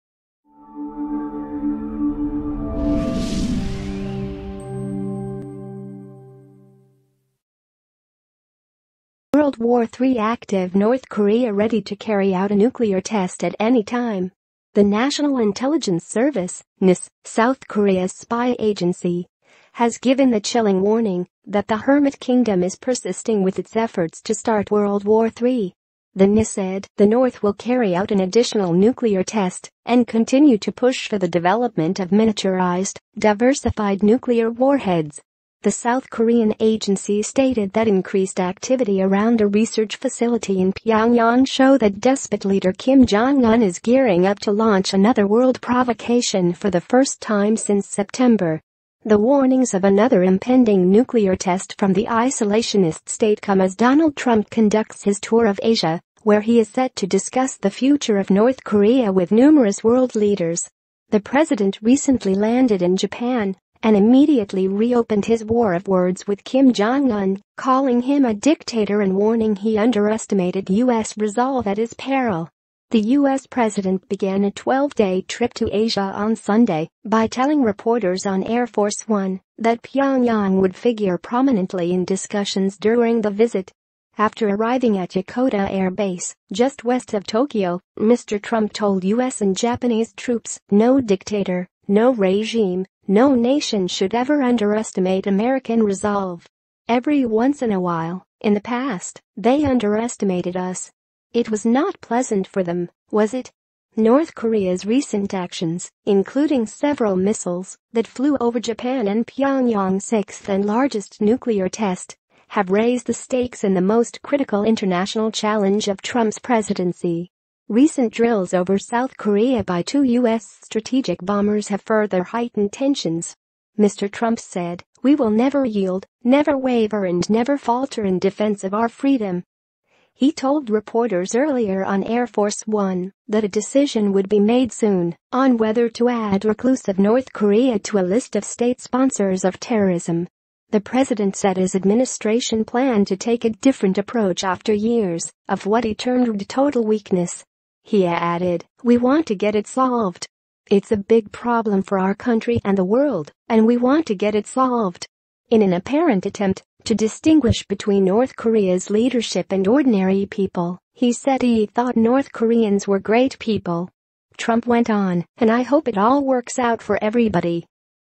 World War 3 'Active' North Korea ready to carry out a nuclear test 'at ANY TIME'
The National Intelligence Service (NIS), South Korea’s spy agency, has given the chilling warning that the hermit kingdom is persisting with its efforts to start World War 3.

The NIS said: "The North will carry out an additional nuclear test and continue to push for the development of miniaturised, diversified nuclear warheads.”

The South Korean agency stated that increased activity around a research facility in Pyongyang show that despot leader Kim Jong-un is gearing up to launch another world provocation for the first time since September.